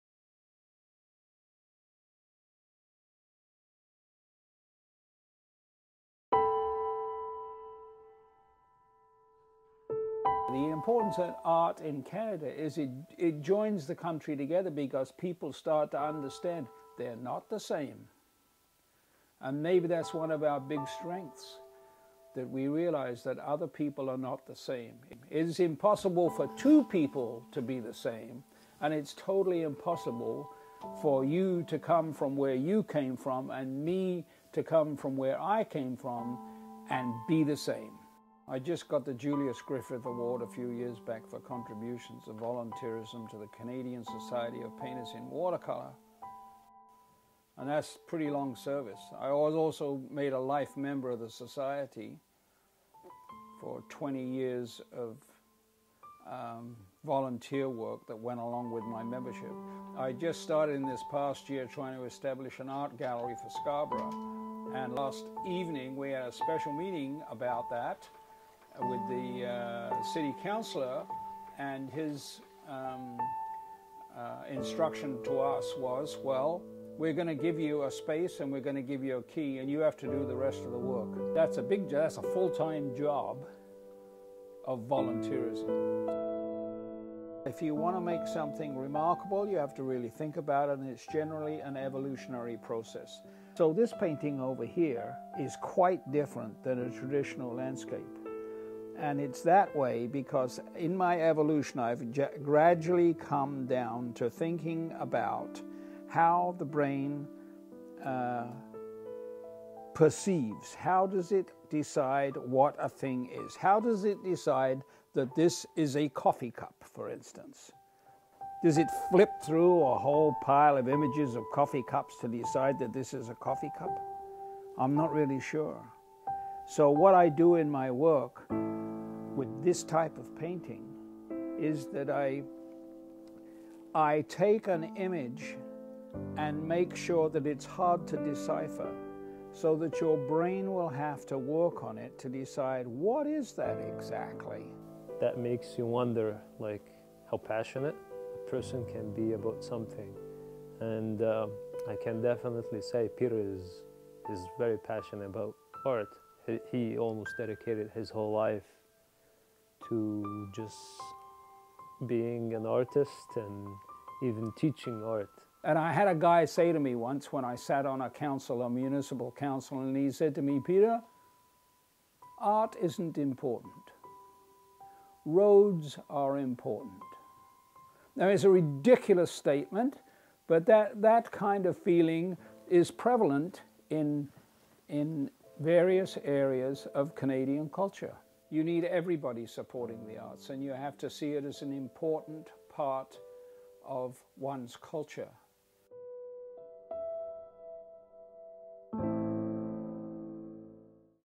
Everyday Hero: Peter Marsh
This is part of an assignment, submitted by Zahraa Al-Akhrass.

Peter Marsh is a Canadian artist and art teacher who dedicated his life for art.
Now he is working on opening an art gallery in Scarborough, ON.

In this short documentary Peter talks about the project, his volunteer work in art, and the importance of art to our lives.